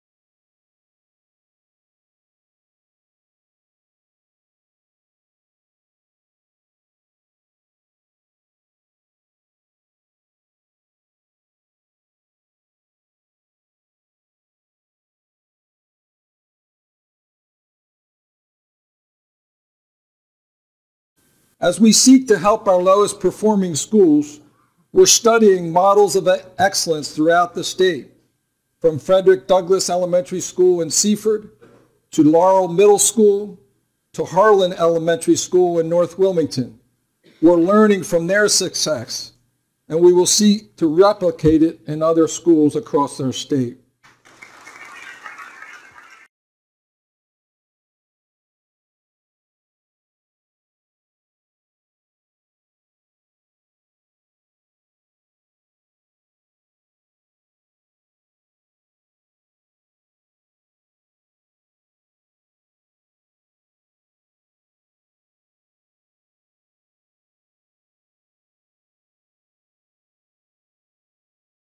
Harlan State of the State 2018
Harlan Elementary mentioned as Model of Excellence in Gov. John Carney's 2018 State of the State Address.